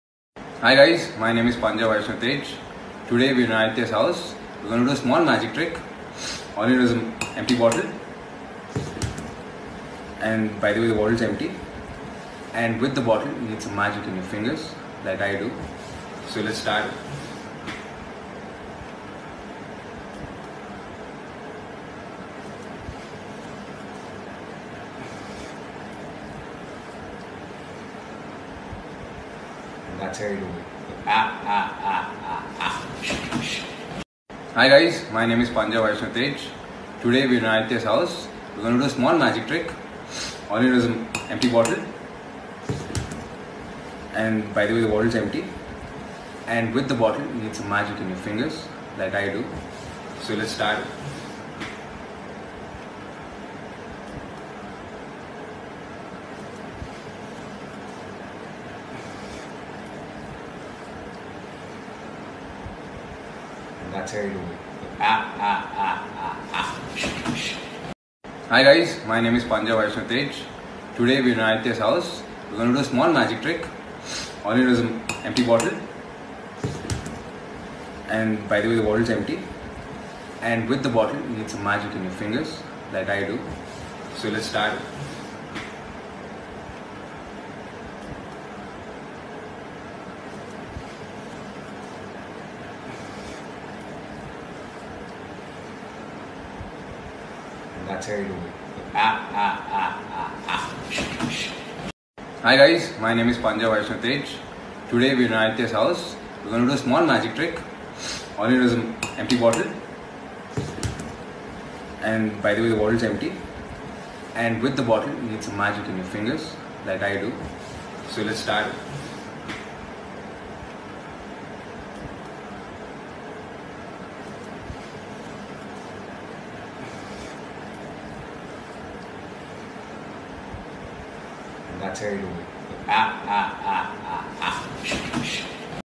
మెగా అల్లుడు మామూలోడు కాదు || Panja Vaishnav Tej Wine Bottle Magic Trick || Movie Blends
మెగా అల్లుడు మామూలోడు కాదు || PanjaVaishnavTej Wine Bottle